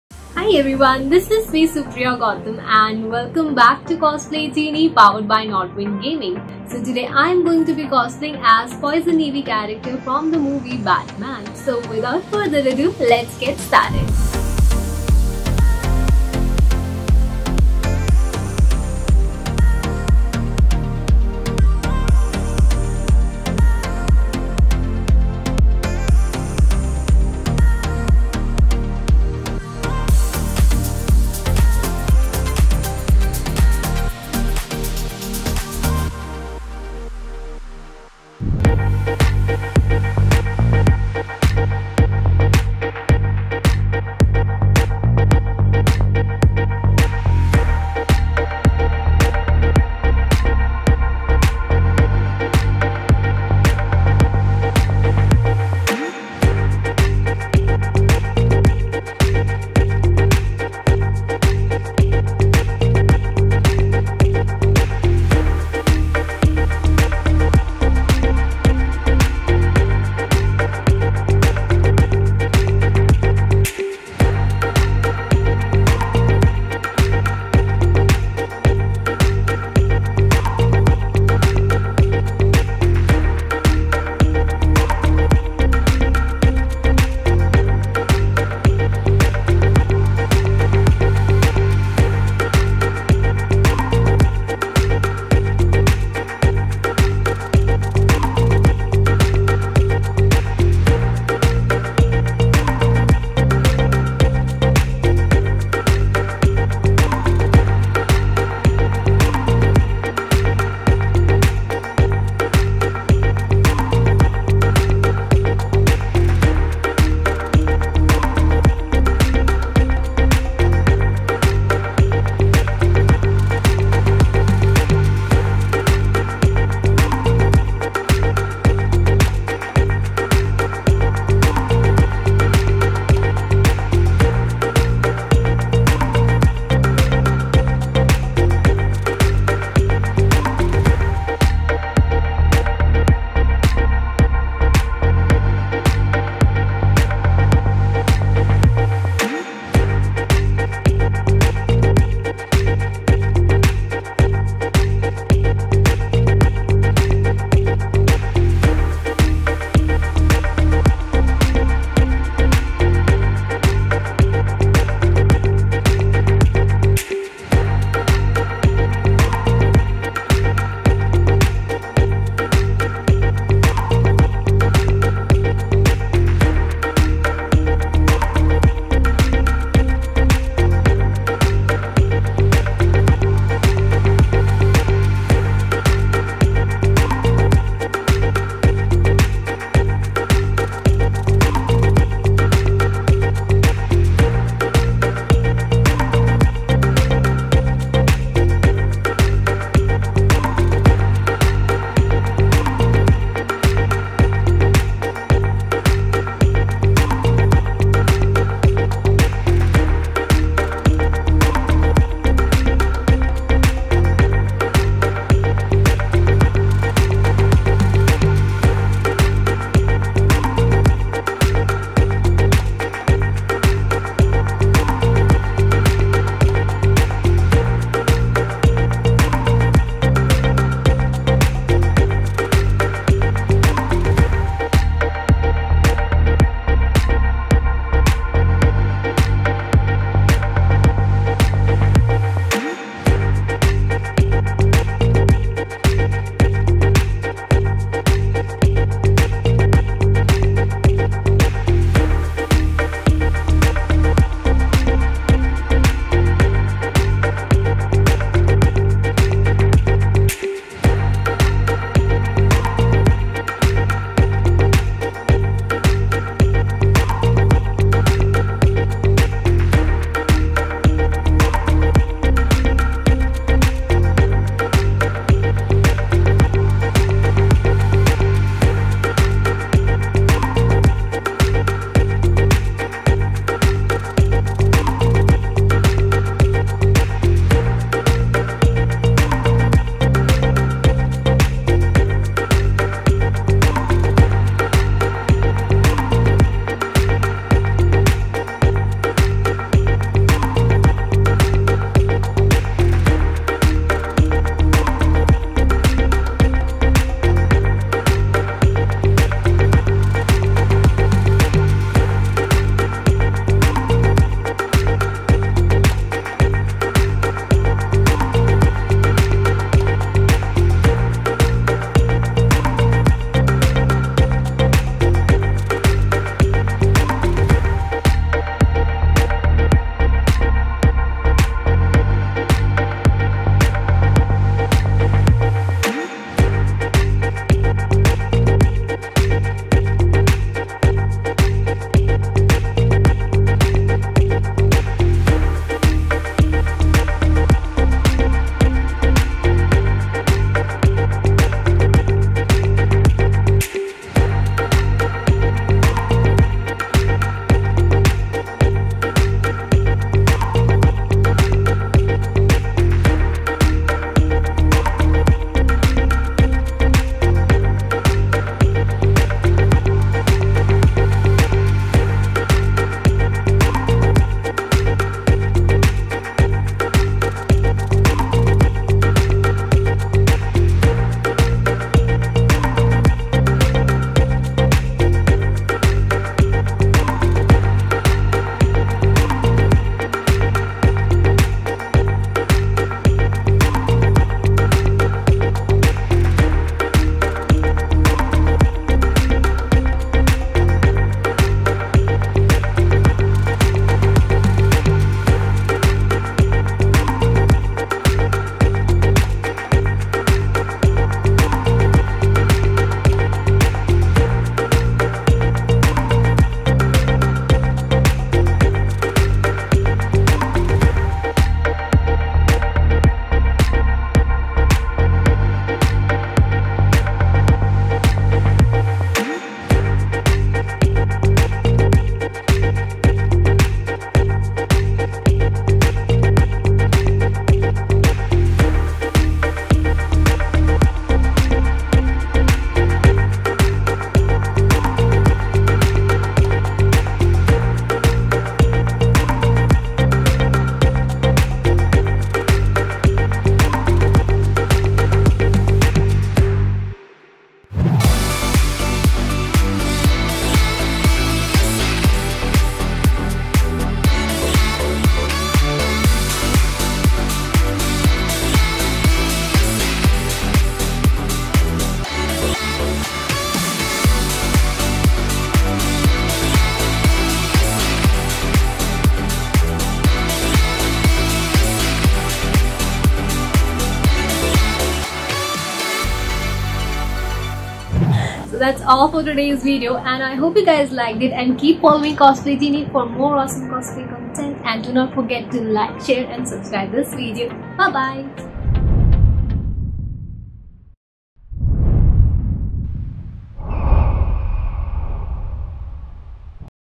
Poison Ivy Cosplay Tutorial | Easy Cosplay DIY
Learn how to cosplay the amazing Poison Ivy with our easy DIY Tutorial.

Materials Required:
1. Green artificial leaves
2. Green sequins
3. Adhesive
4. Red tube top
5. Green shoe
6. Green face paint
7. Eyeliner
8. Eyeshadow
9. Lipstick
10. Massacara
11. Blusher
12. Makeup brush